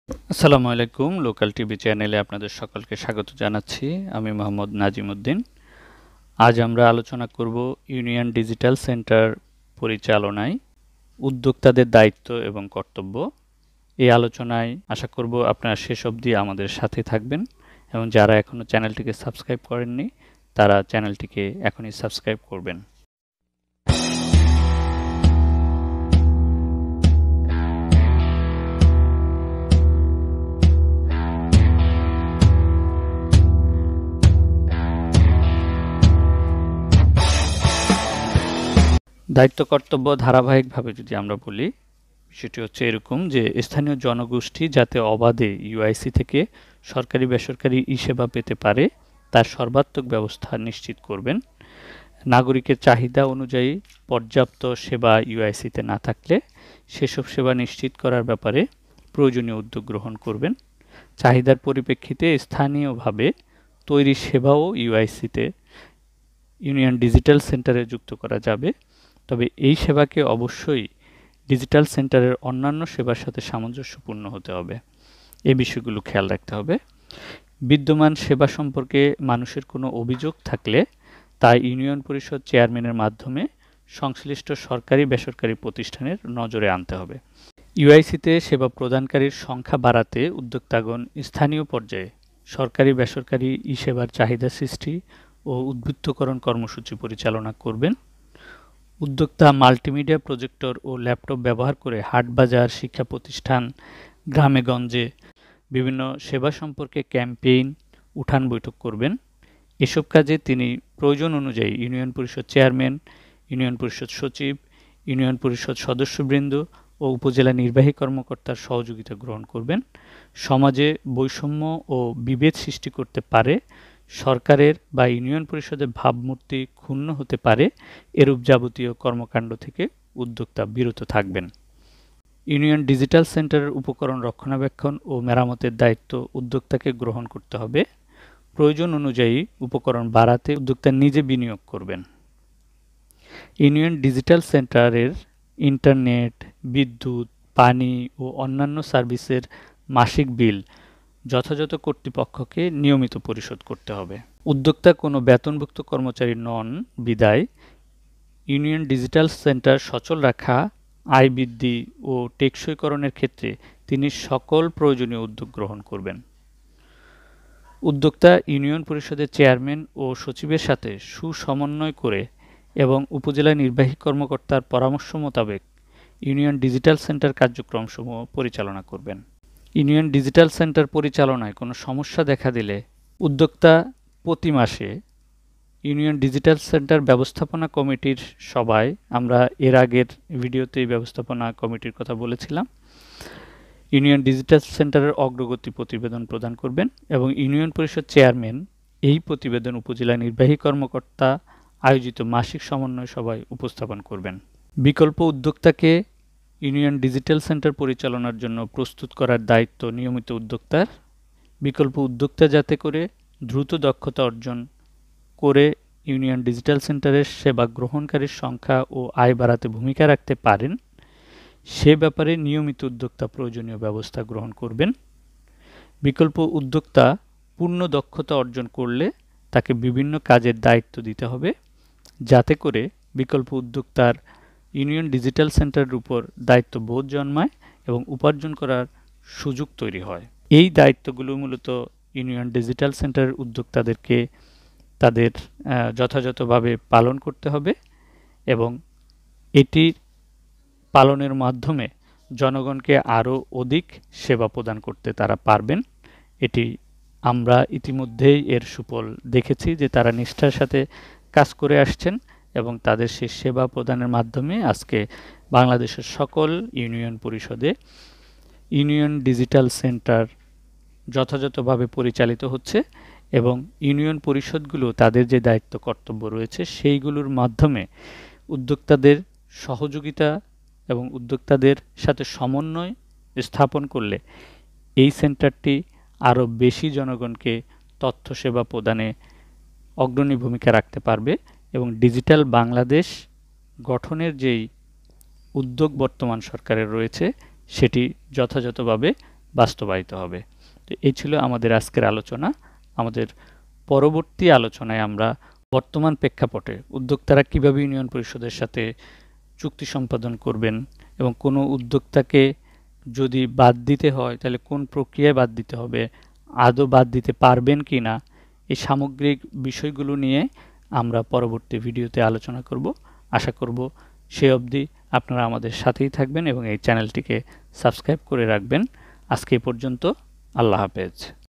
ইউনিয়ন ডিজিটাল সেন্টার। ইউনিয়ন পরিষদ। ইউডিসি। ইউডিসি উদ্যোক্তা। LOCAL TV
আমরা অনেকে্ই জানিনা ইউনিয়ন পরিষদ ডিজিটাল সেন্টারের উদ্যোক্তাগণের দায়িত্ব ও কর্তব্য কি? তাই এই ভিডিওতে উদ্যোক্তাগণের দায়িত্ব ও কর্তব্য বিস্তারিত ভাবে আলোচনা করা হলো।